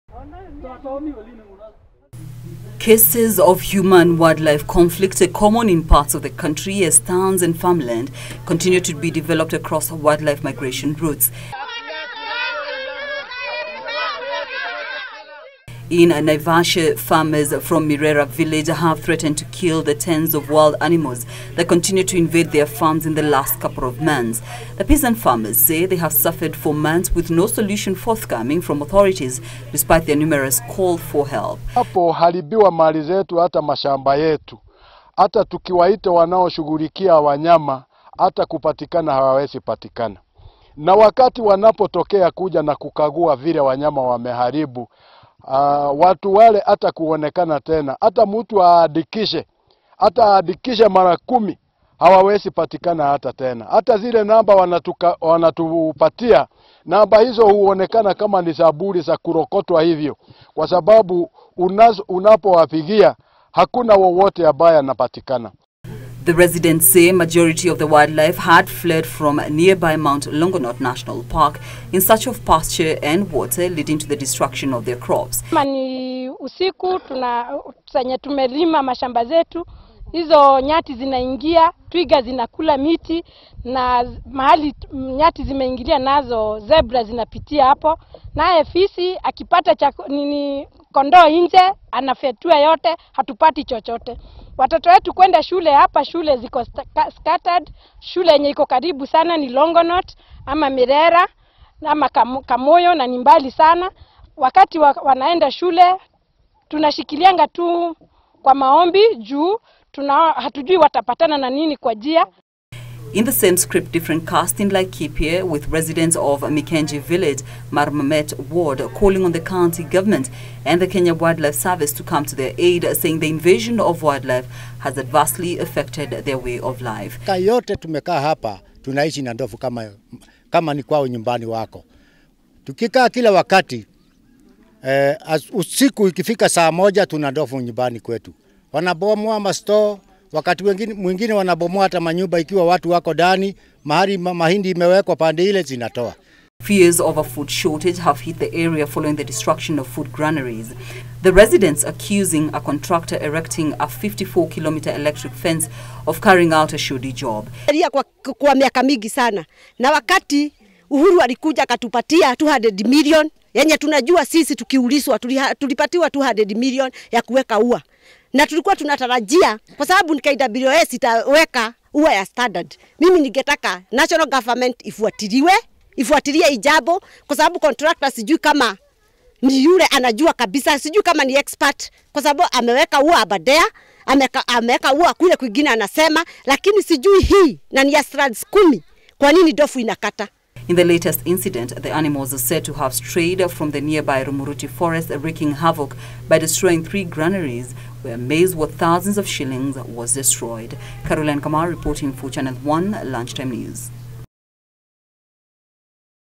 Farmers from Naivasha issue ultimatum to KWS to address human-wildlife conflict
A group of farmers from Naivasha have issued a 14 day ultimatum to Kenya Wildlife Service to address increased cases of human-wildlife conflict failure to which they will take action. The farmers from Mirera village threatened to kill the tens of wild animals that have been invading their farms in the recent past.  In Laikipia County, fears of food shortage abound following the destruction of food granaries by marauding elephants.